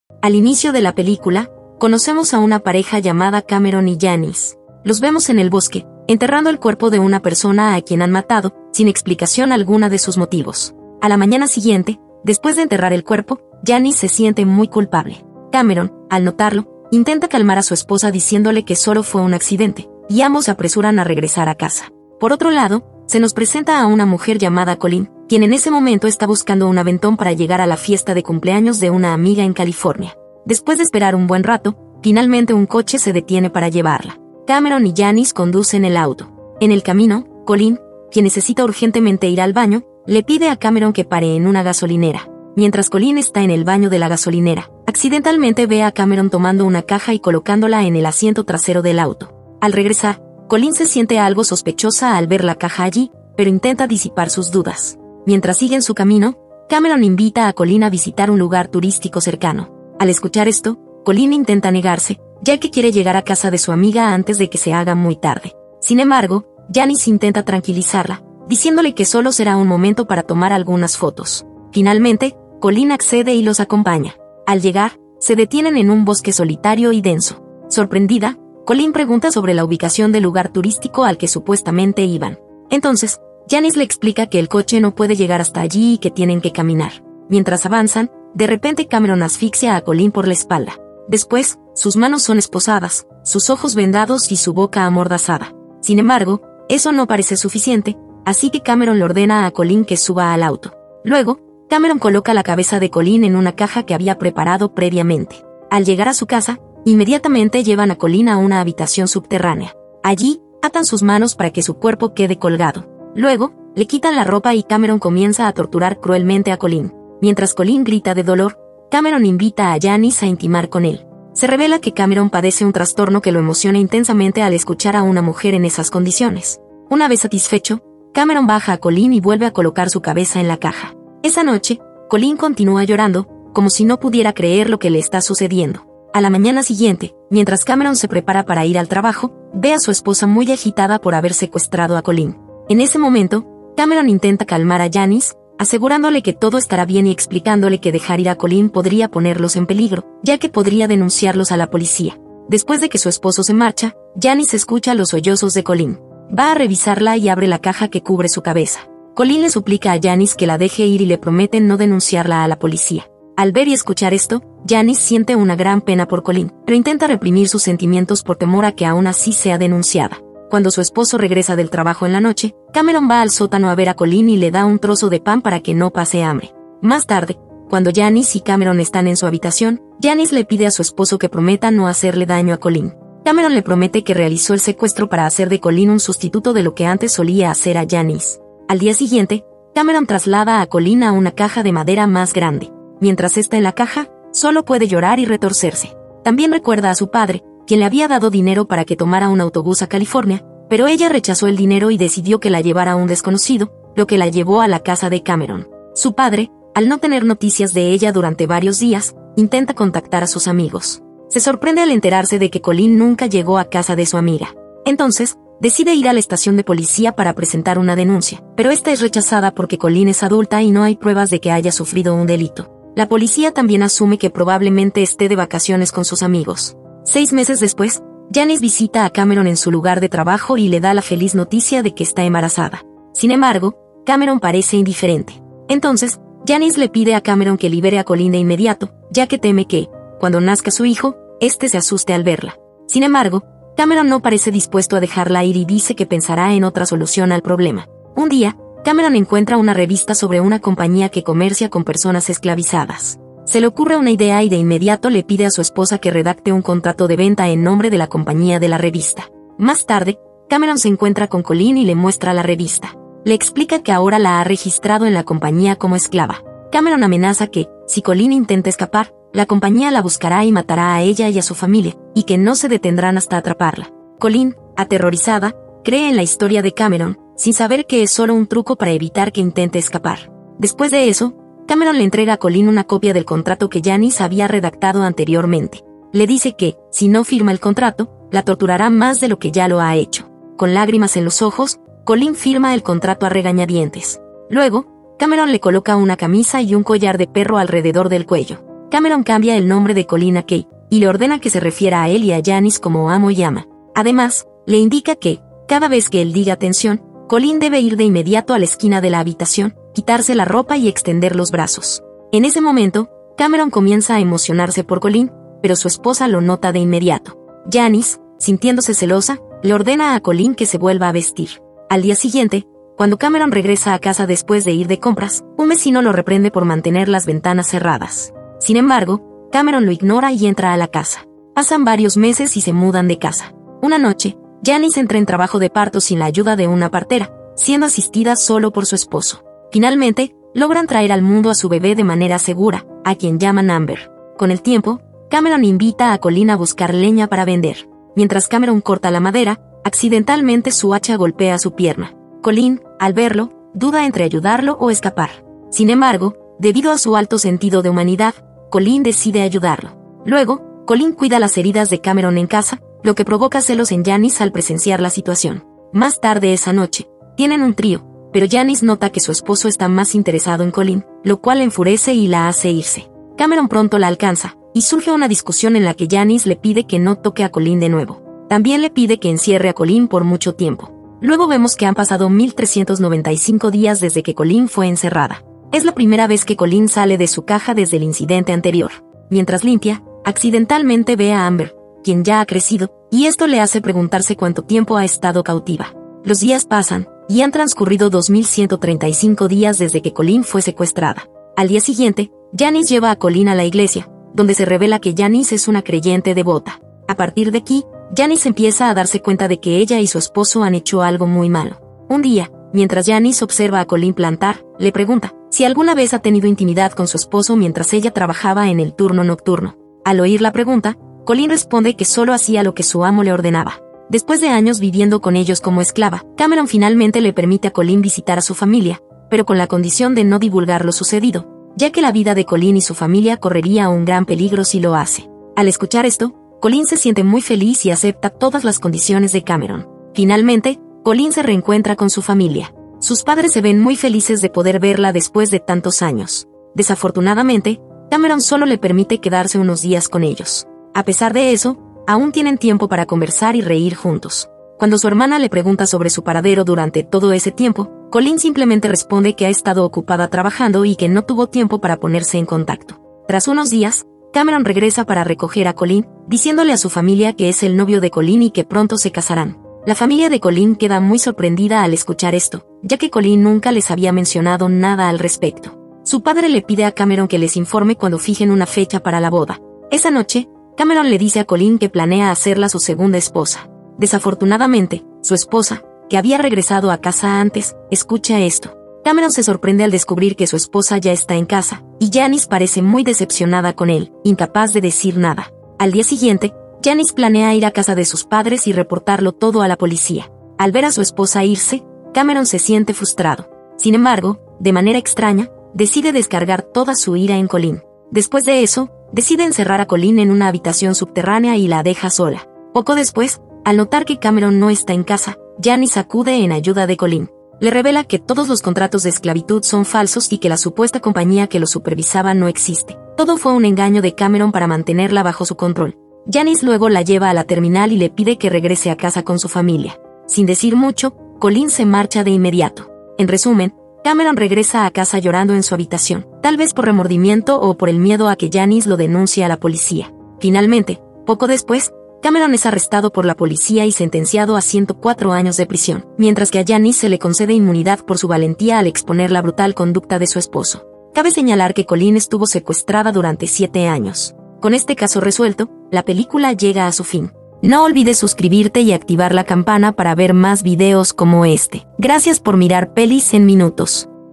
Joven Mantiene A Chica Hermosa Como Su Esclava Durante 7 Años, Y Esto Sucede
Título en los primeros 60 segundos del video.